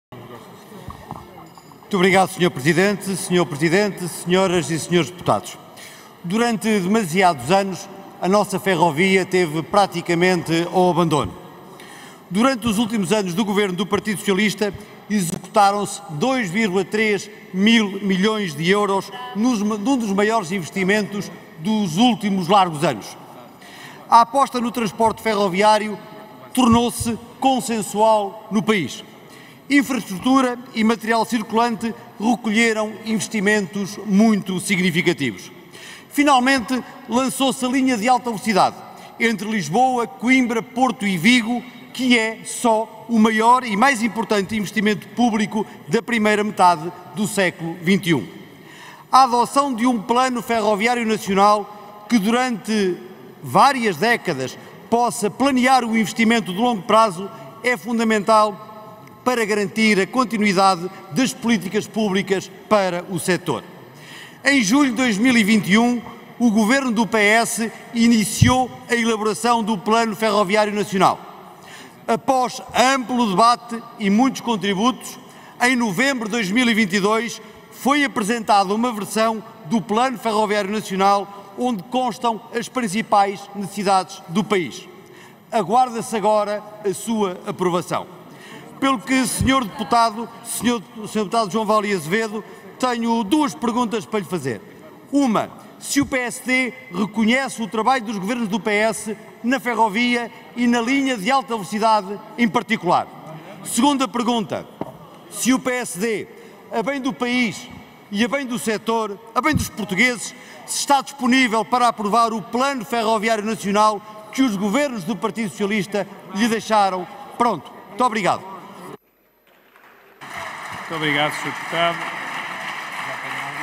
03-07-2024 - Debate Parlamentar | Plano Ferroviário Nacional | Pedro Coimbra
XVI Legislatura
Pedido de esclarecimento do deputado Pedro Coimbra ao deputado João Tilly (CH), no âmbito do Projeto de Resolução (PS), que recomenda ao Governo a aprovação do Plano Ferroviário Nacional